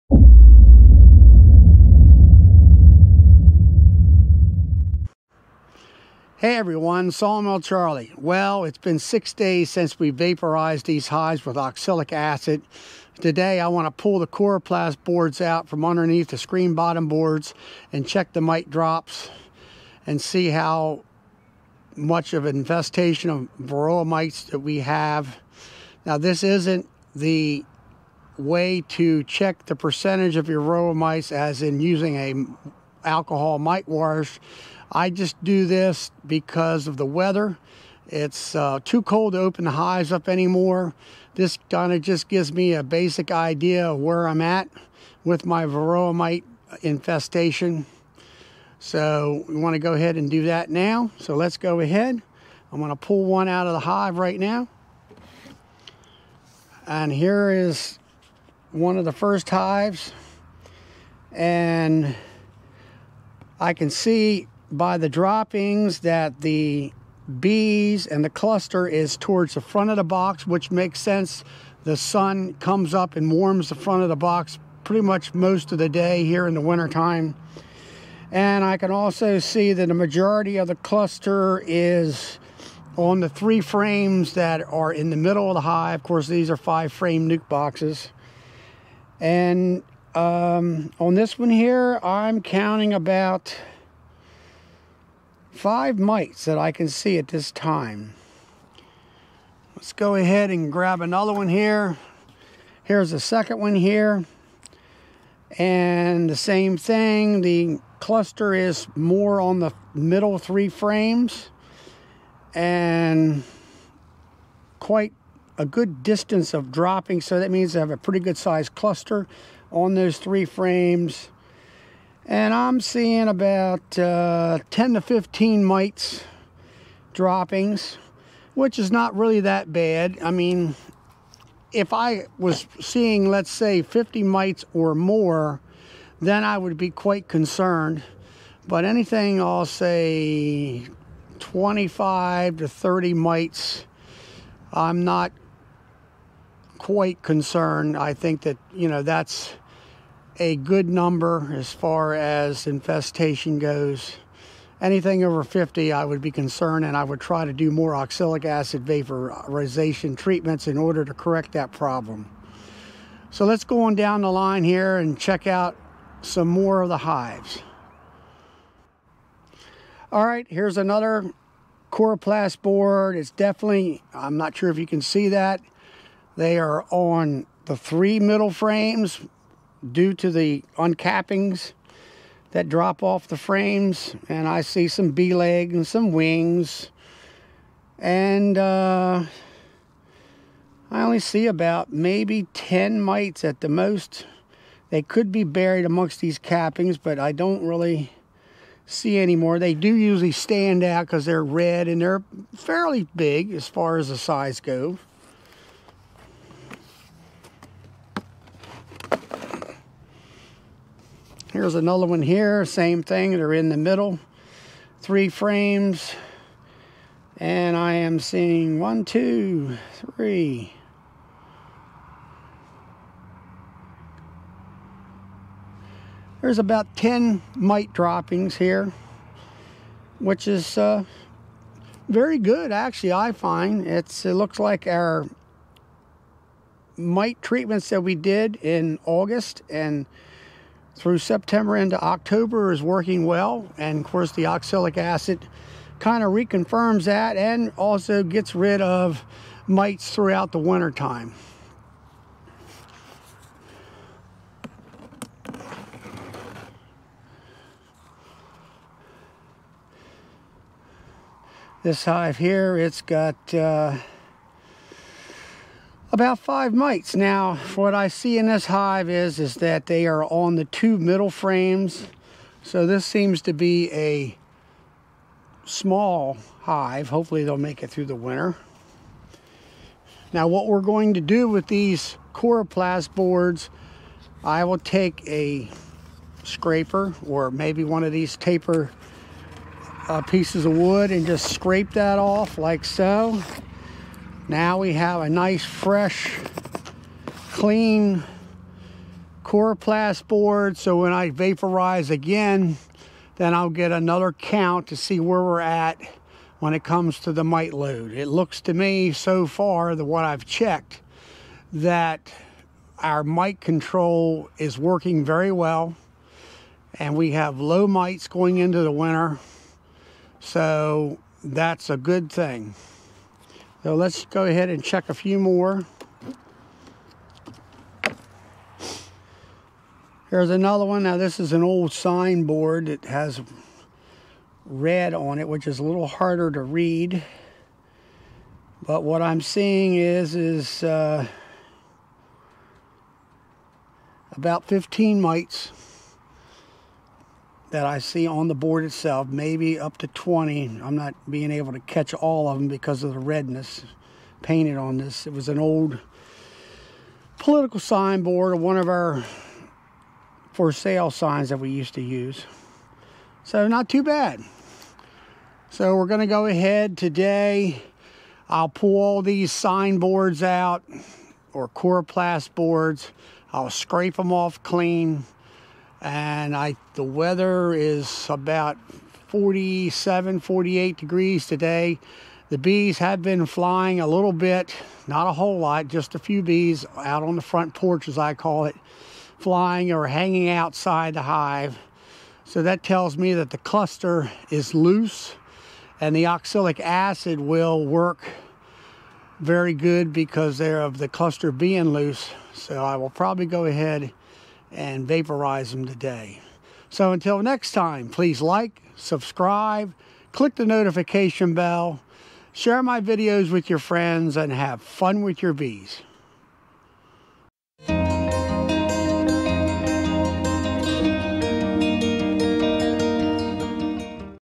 Mite drop check after OA treatment.
In this video I will be going through my hives and pulling the coroplast card board out and checking the amount of mites that have drop after a Oxalic Acid treatment.